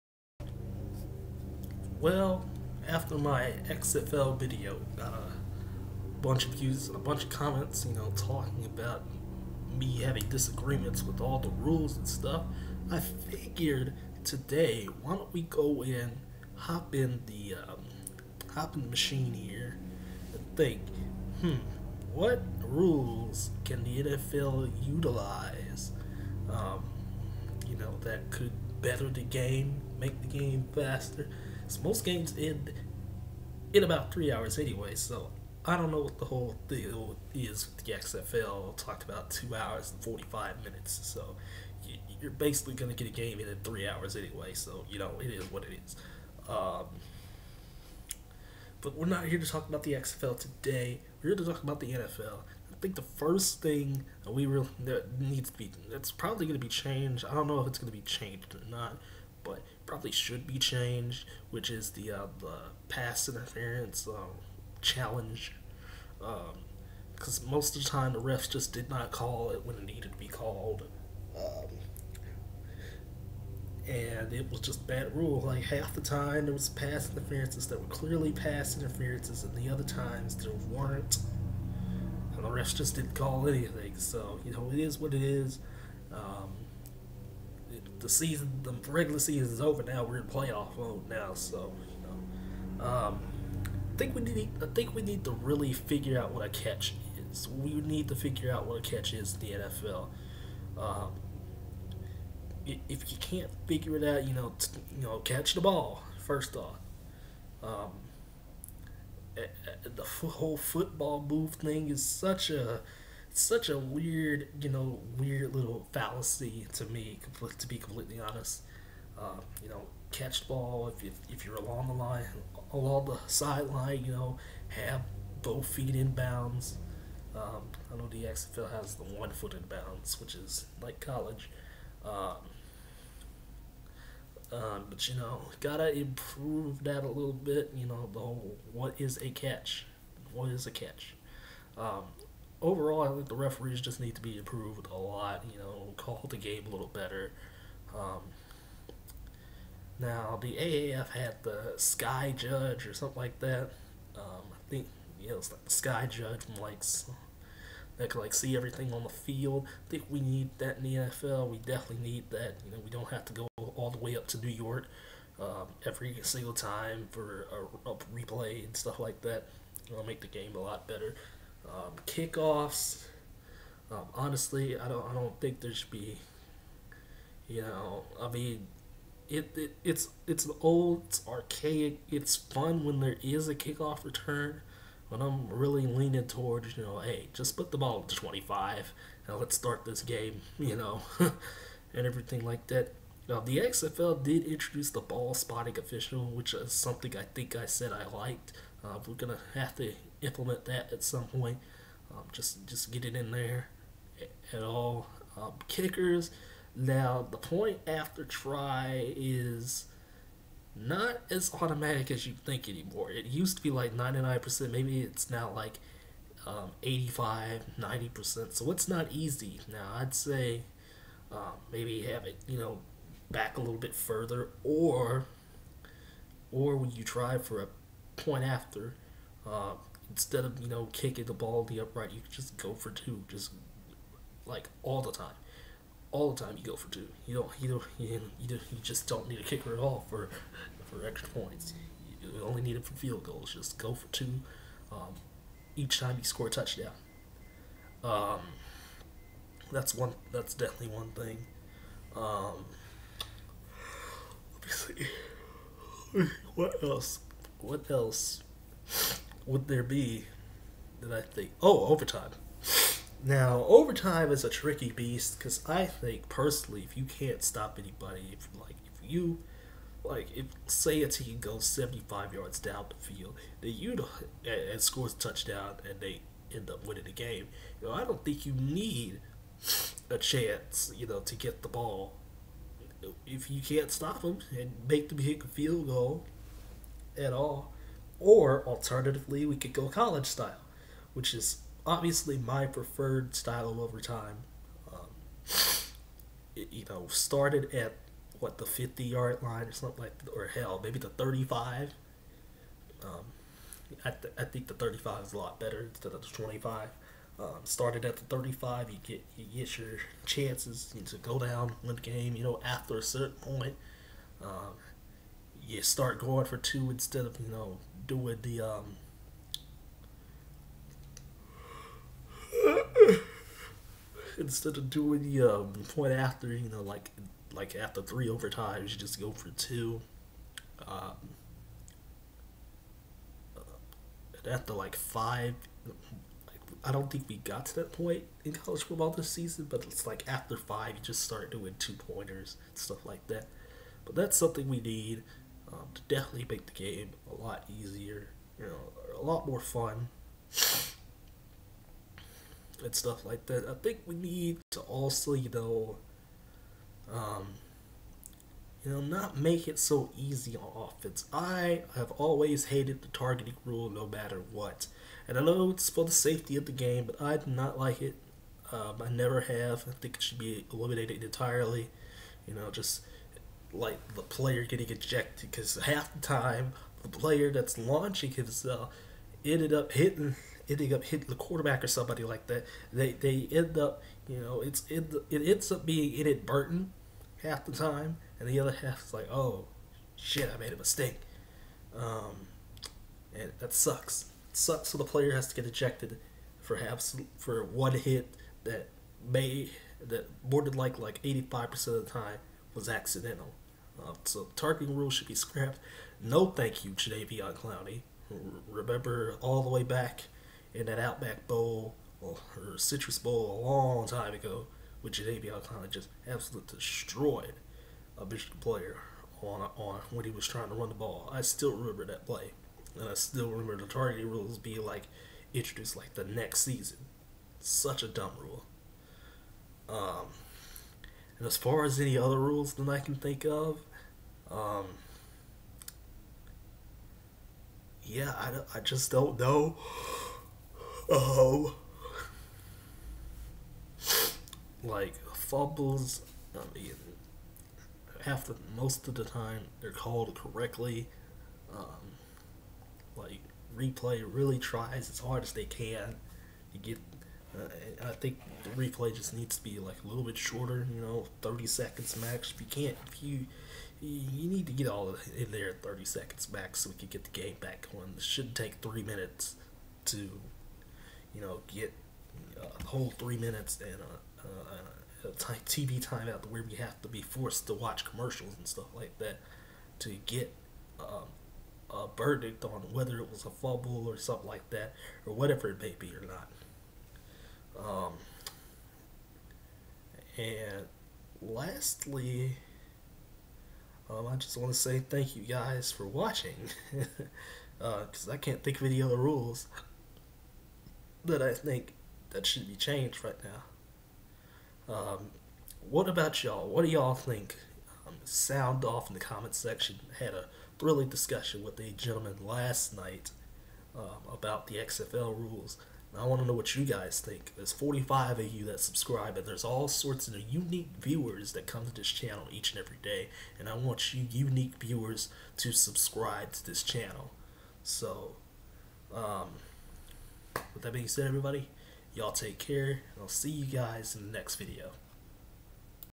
What NFL Rule Changes Do I Think Should Be Implemented?
Thanks to the guy that commented on my XFL video yesterday. Inspiration.